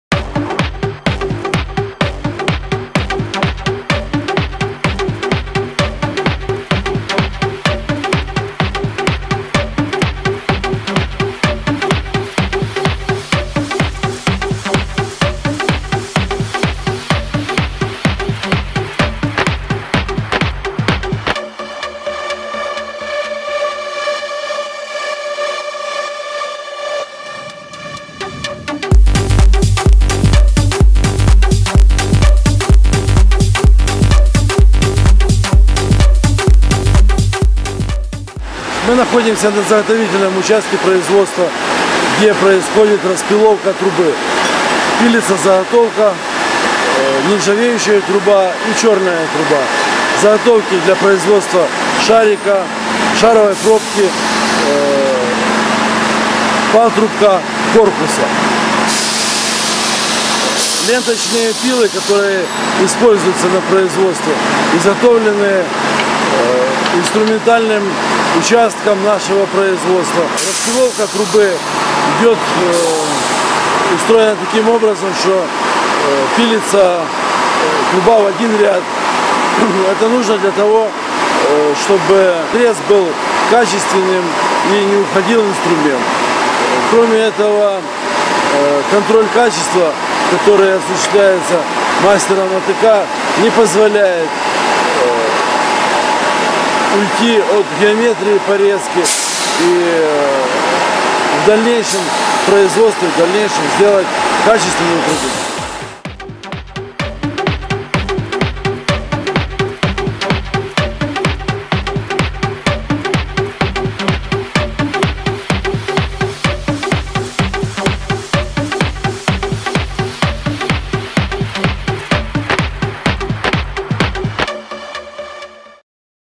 Завод Олбризсервис. Заготовительный участок предприятия от ПТА Armtorg.ru
Открывая срию видеорепортажей о производстве шаровых кранов на заводе "Олбризсервис" мы бы хотели начать с пожалуй основного показателя производства - с заготовительных операций изготовления шаровой арматуры BREEZE. В настоящее время средняя трудоемкость заготовительных работ в машиностроении составляет порядка 40-45% от общей трудоемкости производства. Объясняется это тем, что любое изделие состоит из множества частей. Шаровый кран состоит из - патрубков, корпуса, шаровой пробки, фланцев, маховика(ручка), уплотнительных колец(элементы уплотнения в т.ч. множество доп.уплотнений в виде резиновых и PTFE-деталей), штока и др.деталей.

Конечно на многих предприятиях сегодня развита кооперация, которая дает свободу в выборе поставщиков и ценовых предложений. Но когда имеется собственное заготовительное производство - оно дает гораздо большие преимущества, в условиях нынешней действительности. Во многом это обьясняется сокращением сроков поставки комплектующих, постоянным контролем качества без отрыва от основной программы производства. И конечно же собственное заготовительное производство дает снижение себестоимости комплектующих при проведении оптимизации производственных процессов.

Как мы уже писали, на заводе "Олбризсервис" присутствует полностью собственное заготовительное производство, которое дает преимущества по контролю качества и срокам изготовления. В представленном видеоинтервью директор по производству завода "Олбризсервис", Щербак Александр Владимирович, расскажет о начальных этапах распиловки трубных пакетов и дальнейшей обработке данных комплектующих для производства шаровых кранов ТМ BREEZE.